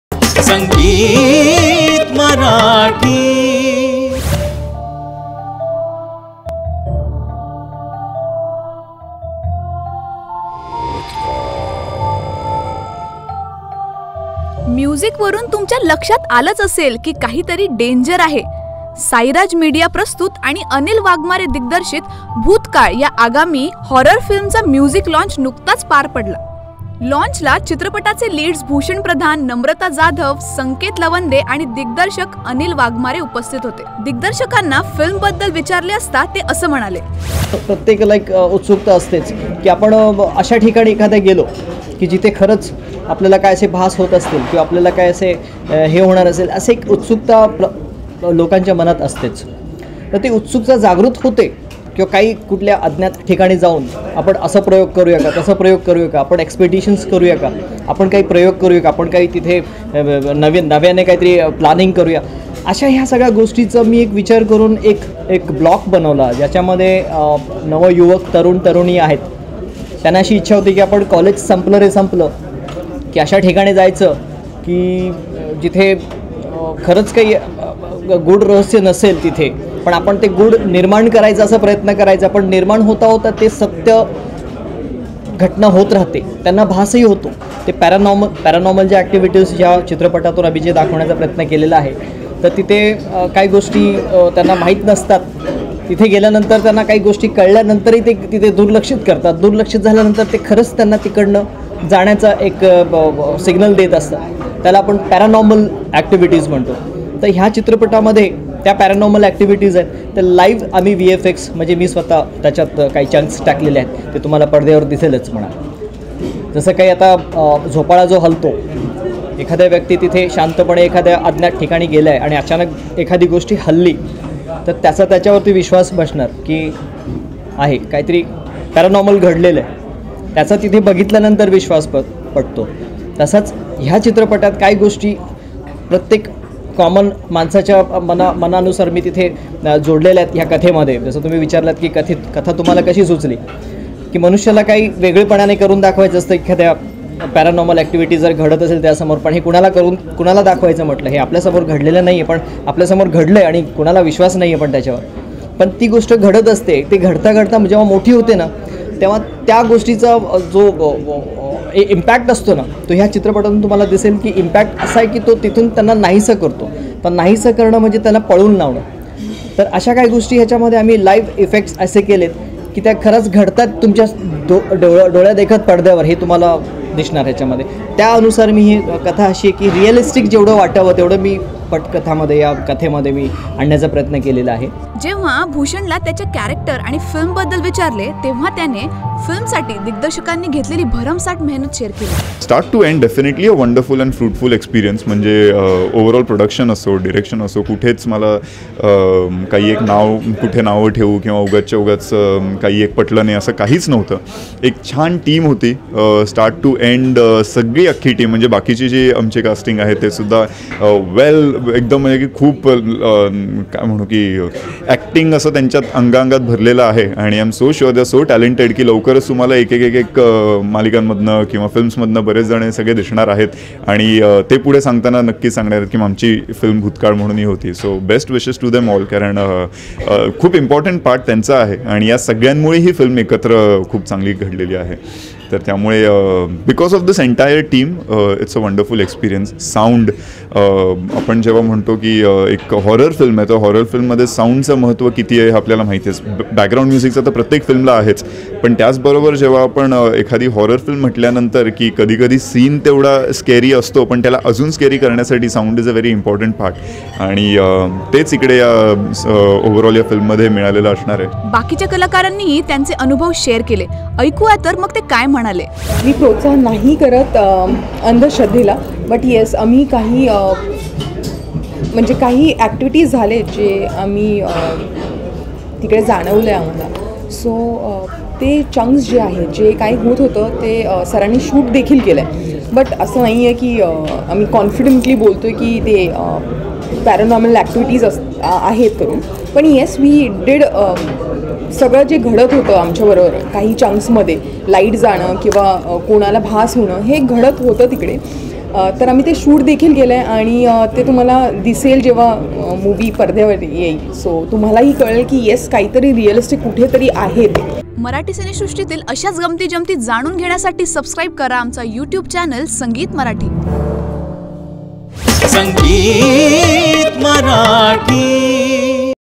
Real or Not Real? | Bhootkal | Anil Waghmare | Sangeet Marathi | Exclusive | 2016
Bhootkal-An upcoming marathi horror film bhootkal's music launch took place and the star cast of the film Bhushan Pradhan,Namrata Jadhav and director Anil Waghmare were present.When asked about the film everyone shared some of their experiences while shooting for the film and even from their real life.